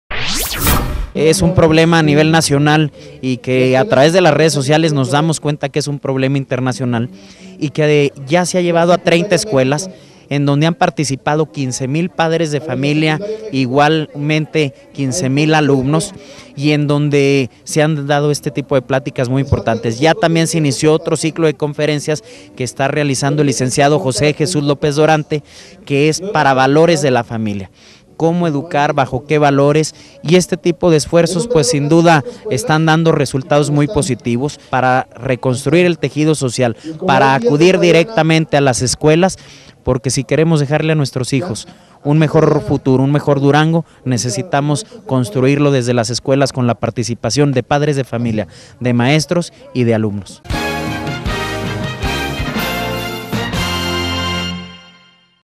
Crecemos promueve valores familiares
Con la finalidad de reconstruir el vínculo entre padres de familia y sus hijos se promueve el programa “Padres exitosos forman hijos ganadores” así lo dio a conocer el delegado de Crecemos, Otniel Garcia Navarro.